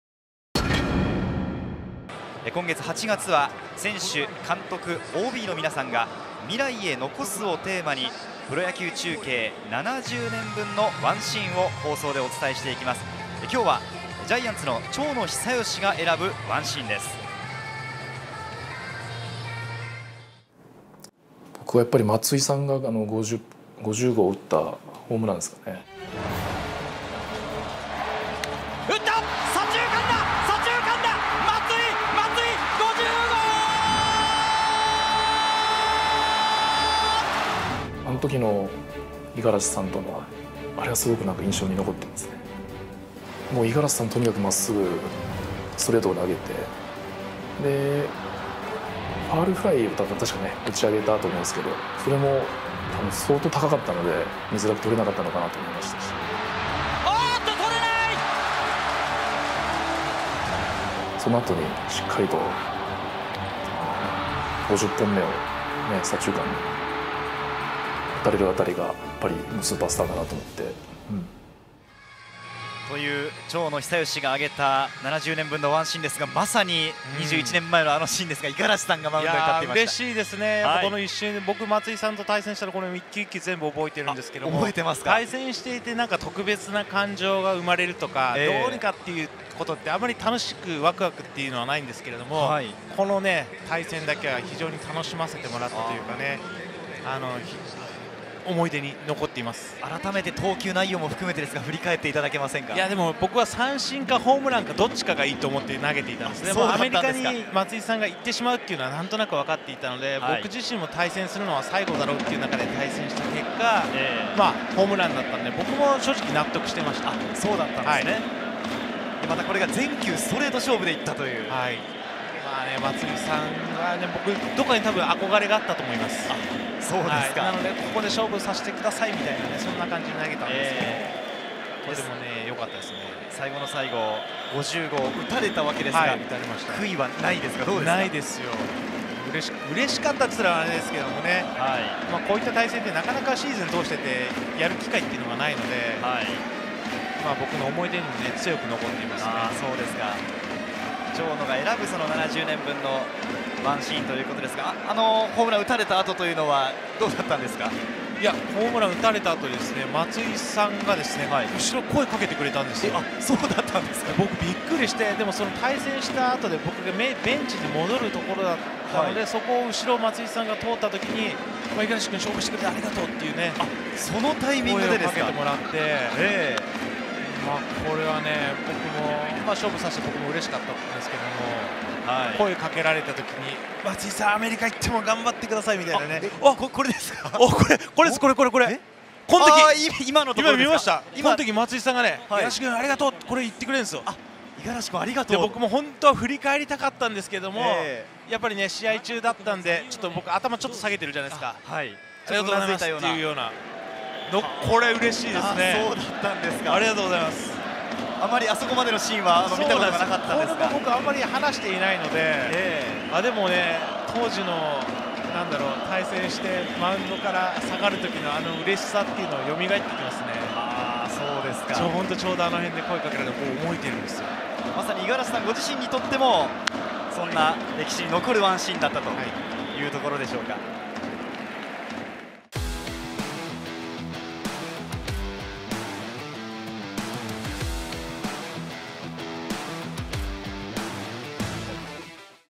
【野球中継70年】長野久義が選ぶ"70年分の1シーン"五十嵐亮太×松井秀喜名勝負秘話
【野球中継70年】
長野久義 が選ぶ"70年分の1シーン"